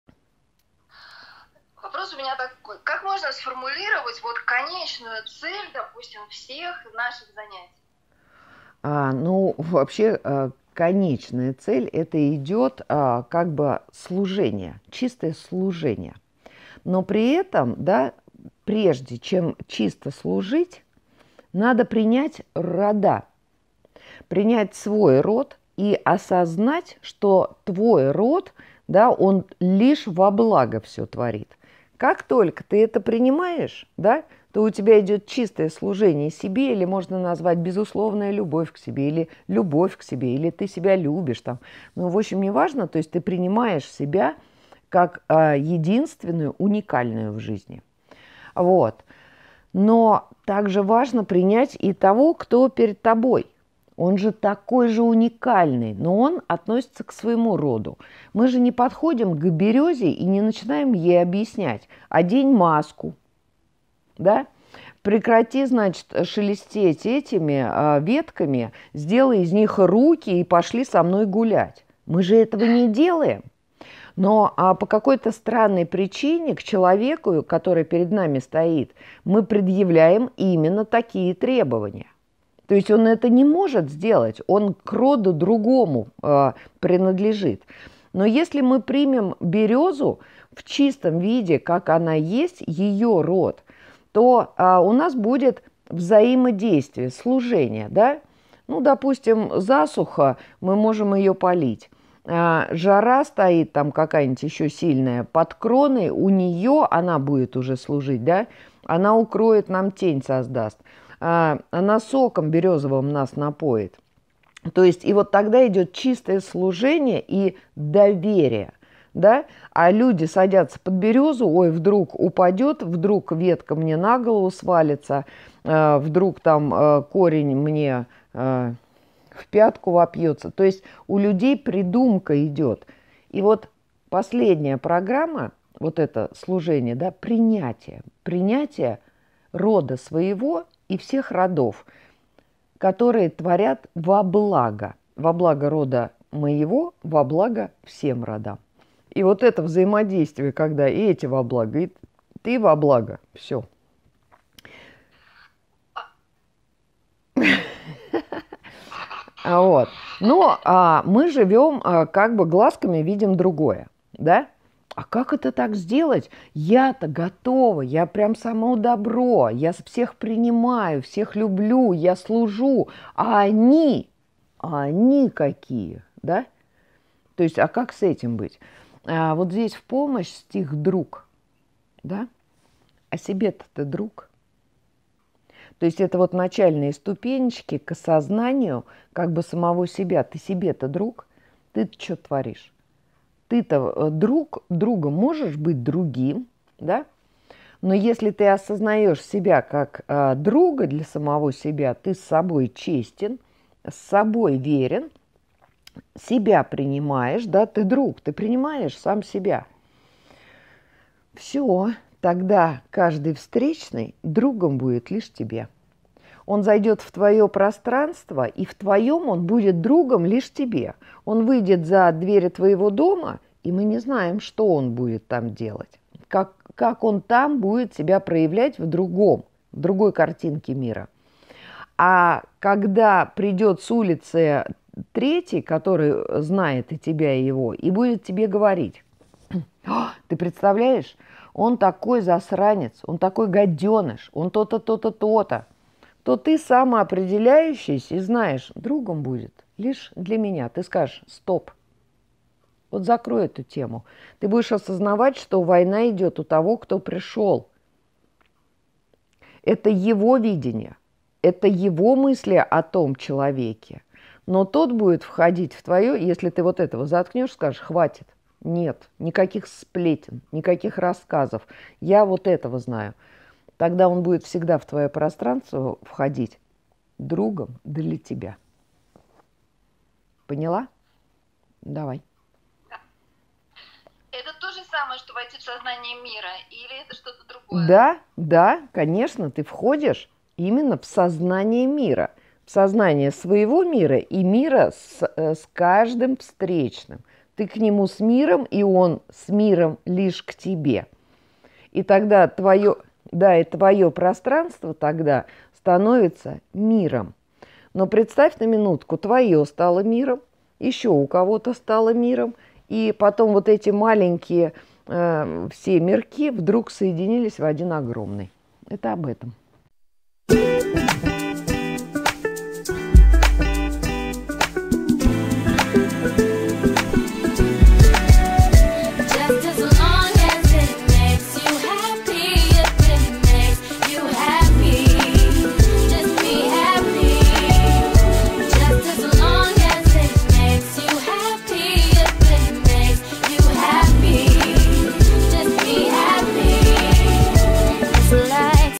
Конечная цель НАШИХ ЗАНЯТИЙ. Какая цель у перехода. Смысл перехода
Конечная цель НАШИХ ЗАНЯТИЙ. Какая цель у перехода. Смысл жизни. Смысл перехода
Зачем заниматься нейтрализацией программ

Благодарим за поддержку канала:

Карта Сбербанка:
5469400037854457